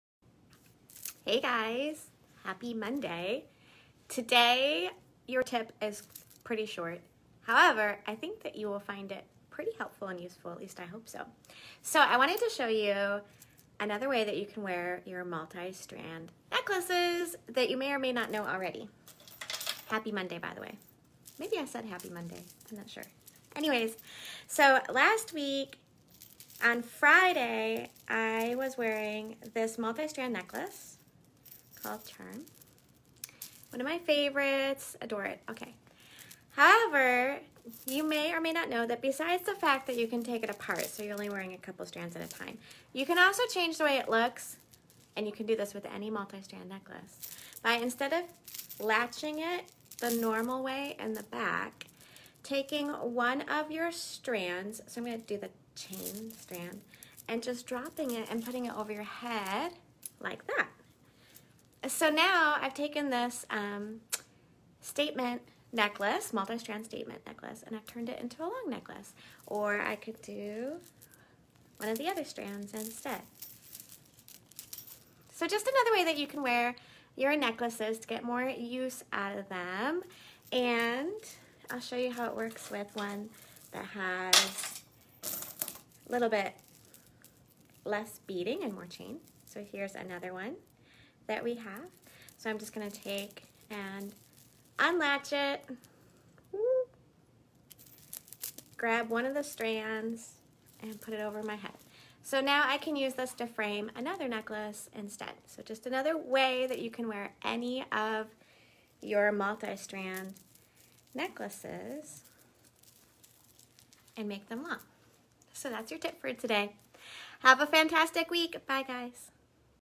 Multi-Strand Necklace Hack - Park Lane Jewelry
You probably already have multi-strand, beaded necklaces in your jewelry collection. Now you can dig them out and wear them a new way for even more looks!

Want to catch me LIVE with my styling tips featuring Park Lane Jewelry?

Love these looks?